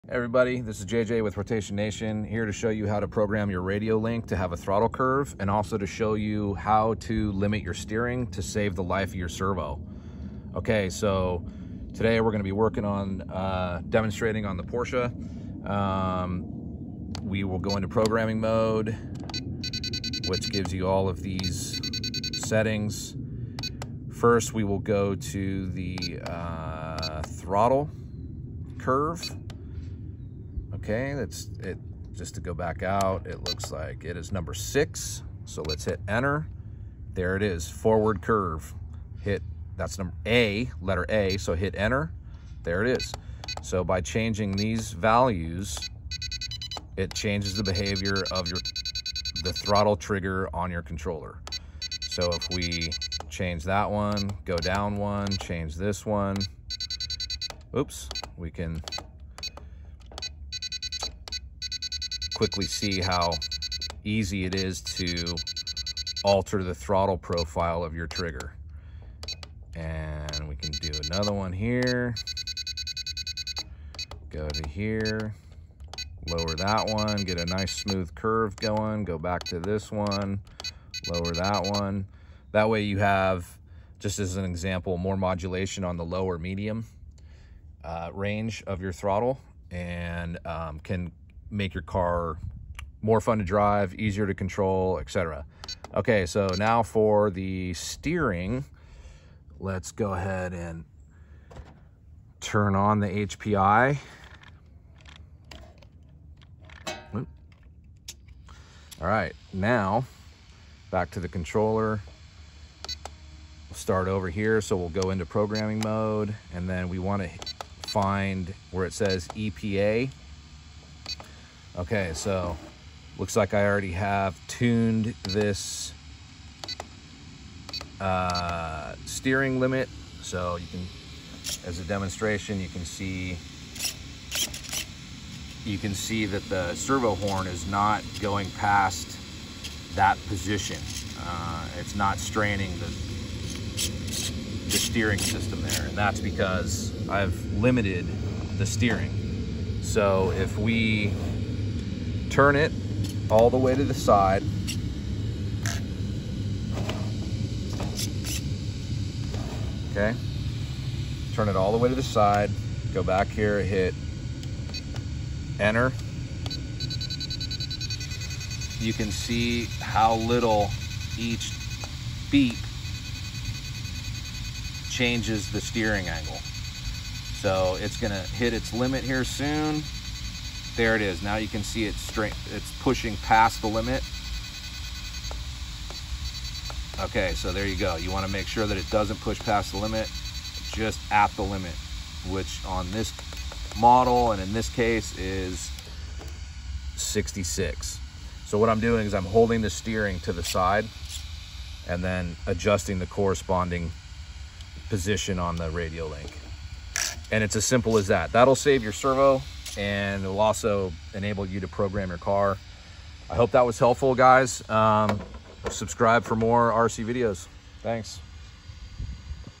Radiolink Transmitter Tuning // Steering Limits // Throttle Curve
Throttle Curve: enter program mode by pressing exit and enter at the same time, find option 6 then option A and tune your throttle curve according.

Steering: enter programming mode and find and enter into EXP. Turn your car on and lower the right or left value until the servo reaches the steering linkage limit.

all done now, charge and go drive!